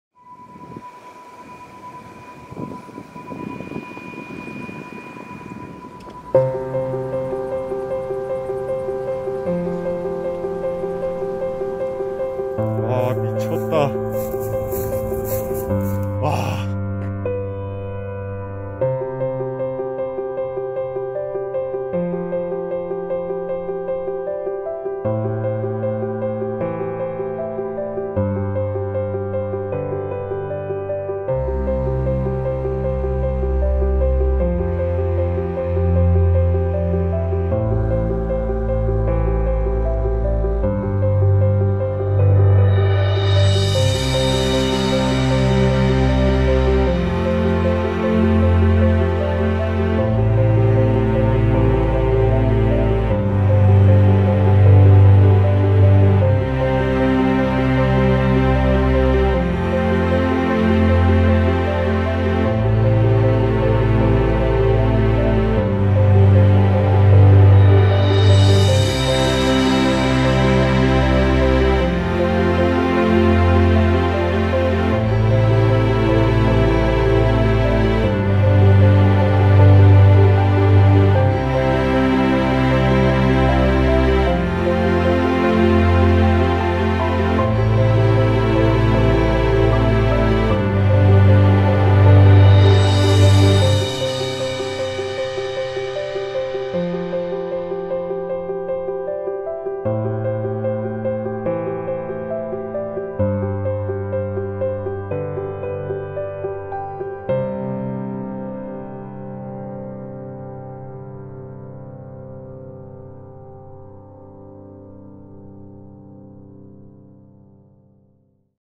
ef 50mm f1.8 VS md 50mm f1.4 Lens Comparison. 50mm 단렌즈로 담아본 육백마지기 풍경 사진 및 영상(가성비 렌즈 vs 빈티지 렌즈)
가성비 ef 50mm f1.8렌즈와 빈티지 md 50mm f1.4 렌즈로 찍은 사진과 영상입니다.
비교를 위해 보정은 전혀 하지 않았습니다.